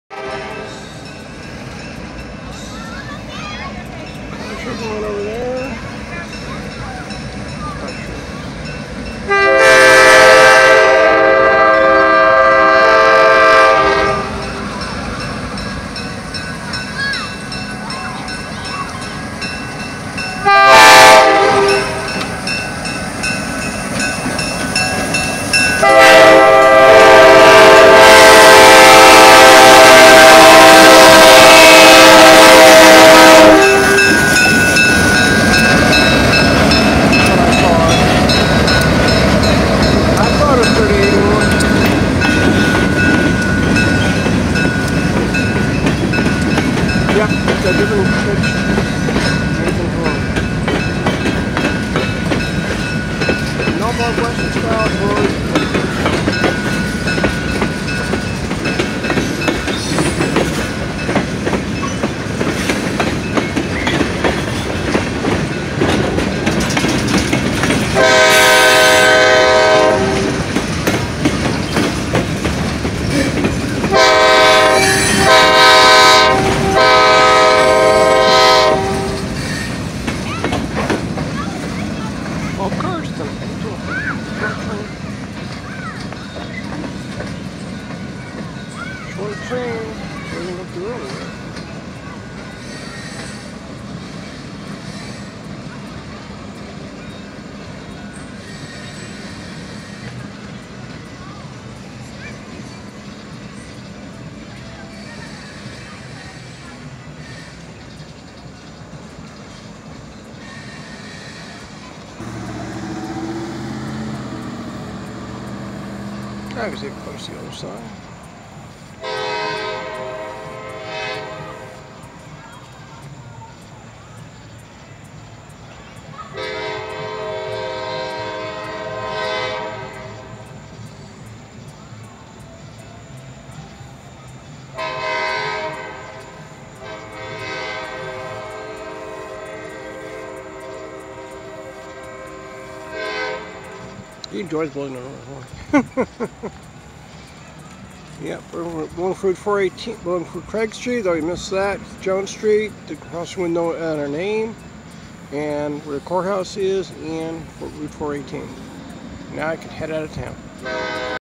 CAROLINA& PIEDMONT Enters FOUNTAIN INN SC During a small car show and rock music!
YEP! THE CAROLINA& PIEDMONT TRAIN COMES INTO FOUNTAIN INN SC. DURING AN EVENT!AS CLASSIC EARLY 🎸 ROCK MUSIC PLAY'S.IT IS SOON OVERTAKEN BY THE SOUND OF 2 645 V16 DIESEL LOCOMOTIVE ENGINES!AND THE OVERALL SOUND OF THE TRAIN CRUZING THROUGH TOWN!WIITH GP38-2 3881 EX UNION PACIFIC,AND GP38 2020 EX GULF MOBILE & OHIO. LEAD 12 CAR'S TOWARDS LAURENS SC! 8/17/17 LORD BLESS Y'ALL! ENJOY!